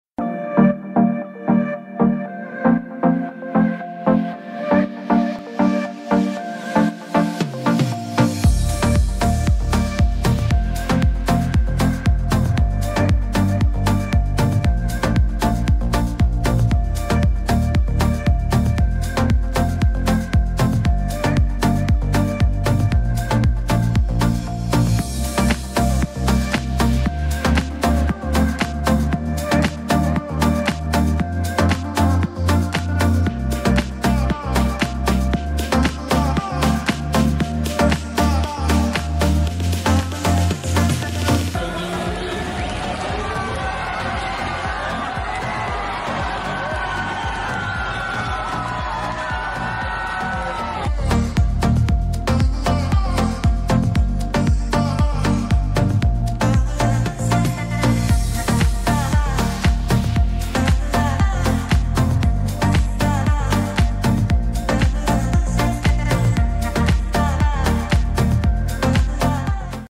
Et la meilleure en Ju-Jitsu est... Marocaine !💪🏻😍🇲🇦👏
Cette Marocaine est 8 fois championne du monde de Ju-Jitsu !💪🏻😍🇲🇦👏